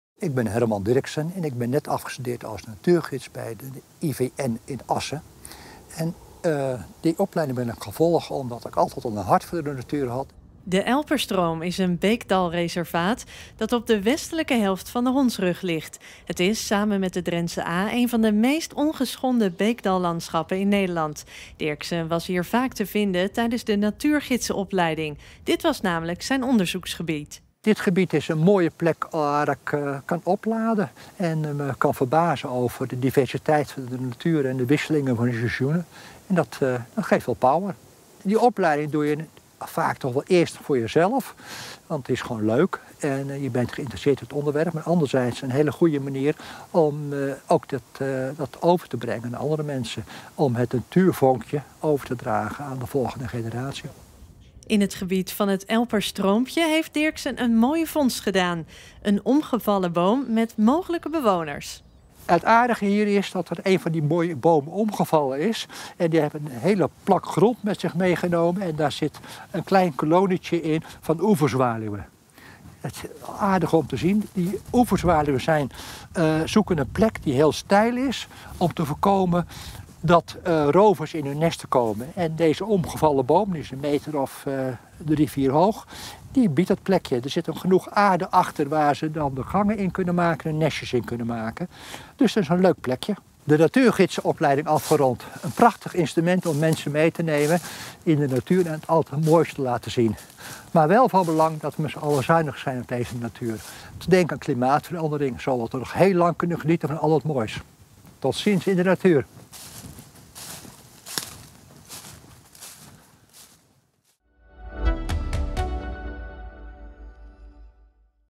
Nieuwe natuurgids in Drenthe: Herman Dirksen
IVN Natuurgids Herman Dirksen: ‘Ik wil het natuurvonkje overbrengen op de volgende generatie’

Ontdek de Drentse natuur op ROEG!